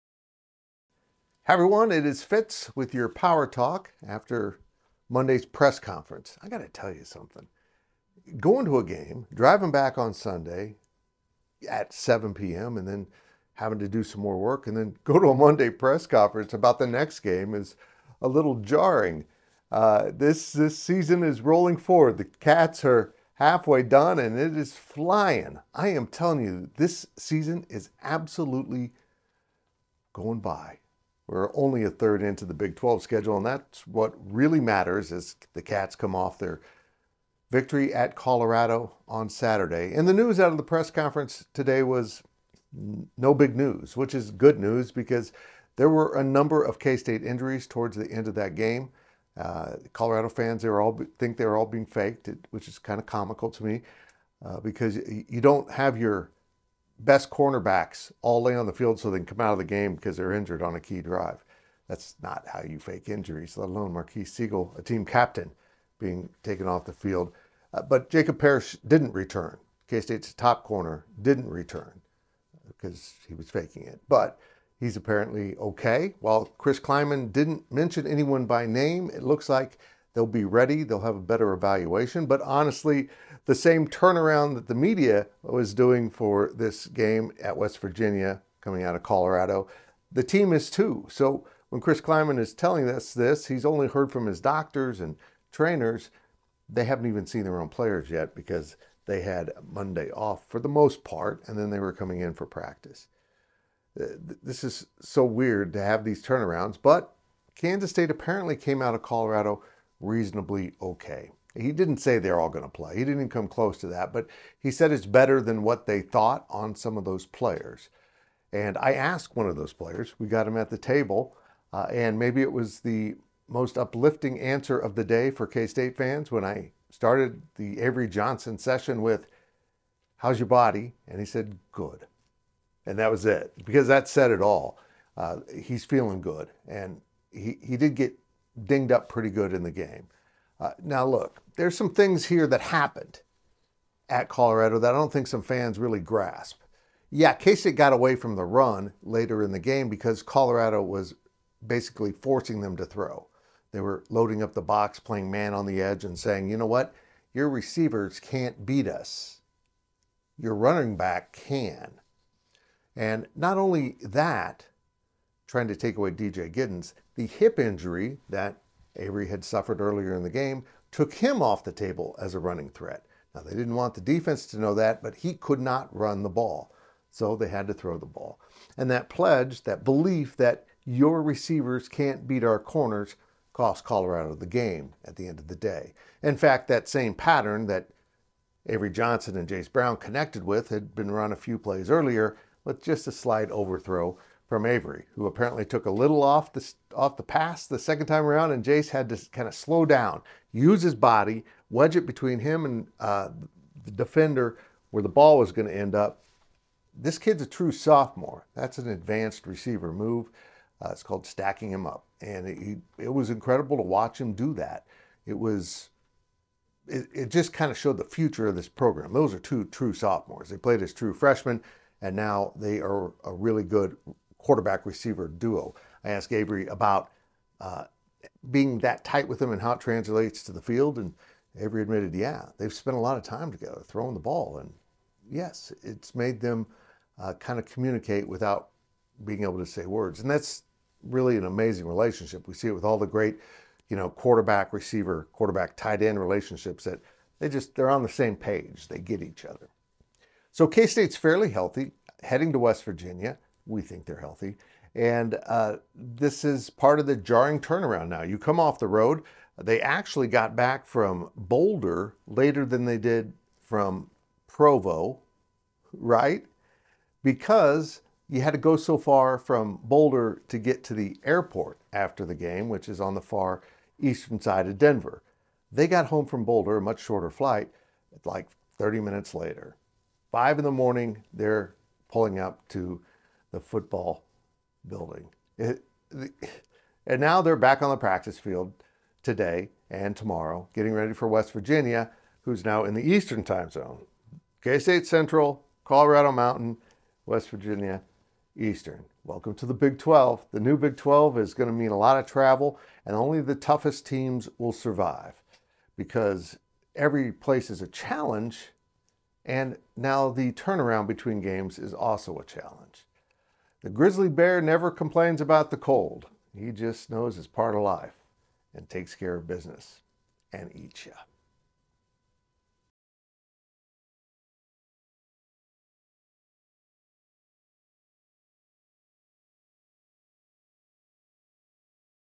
Tighter travel and recovery schedules are now part of life in the Big 12 | Power Talk
POWER TALK | 10.14.24 | Kansas State came out of its 31-28 victory at Colorado on Saturday night pretty banged up. Still, it sounds like there's a good chance all can play this Saturday at West Virginia, including quarterback Avery Johnson, who gave Fitz a precise answer when asked about how he physically feels. After returning home at 5 a.m. on Sunday, the Wildcats are now preparing to play at West Virginia, but farther travel and tighter schedules are now included with membership in the Big 12.